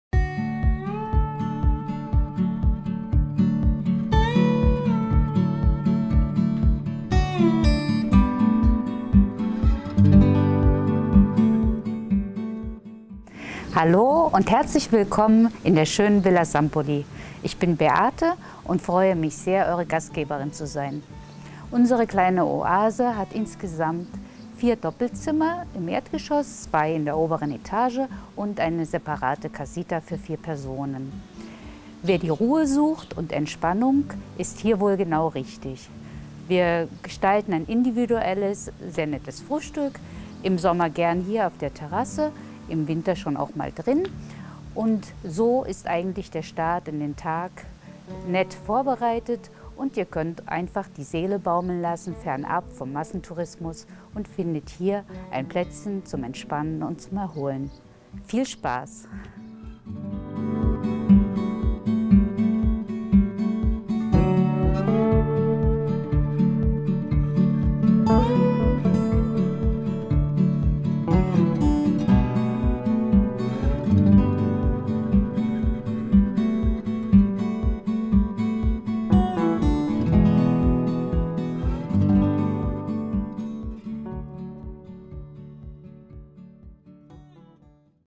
Villa Sampoli
Finca-Hotel Villa Sampoli in Llucmajor auf Mallorca, das etwas andere Finca-Hotel, Adults only